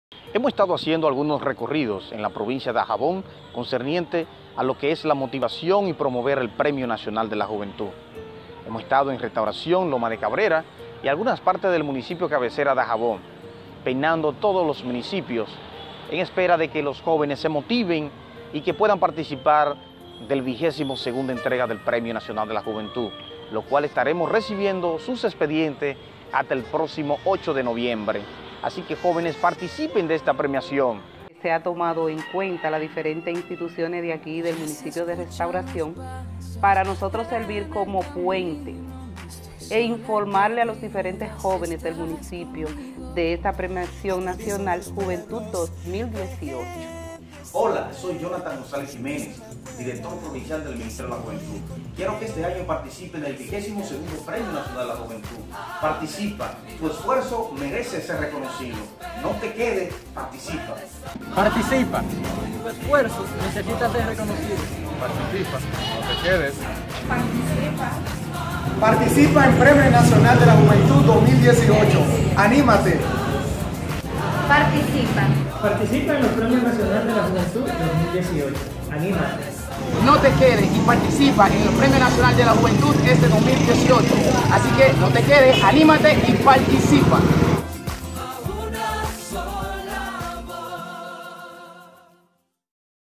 ¡Así hemos estado promoviendo el Premio Nacional de la Juventud 2018!
¡Así hemos estado promoviendo el PNJ2018!🏆¿Tienes el talento y has realizado una labor merecedora de este galardón?¡Anímate a participar!